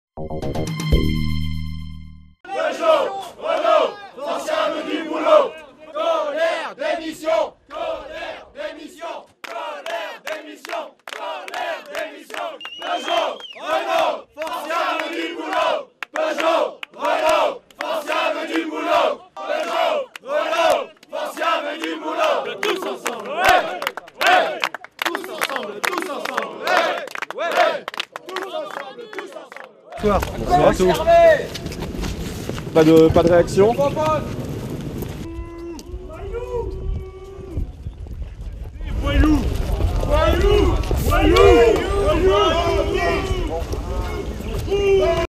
Another 'bossnapping' in France
France witnessed a new "bossnapping" incident late Thursday in which disgruntled factory workers at auto parts maker Faurecia held three managers for five hours. Recent weeks have seen a number of business executives in France held hostage, some overnight, by workers angry over restructuring and lay-off plans. In each case, they have been released unharmed, often after a promise to restart negotiations. Images of manager's release at the Faurecia factory overnight Thursday amid angry crowds of workers.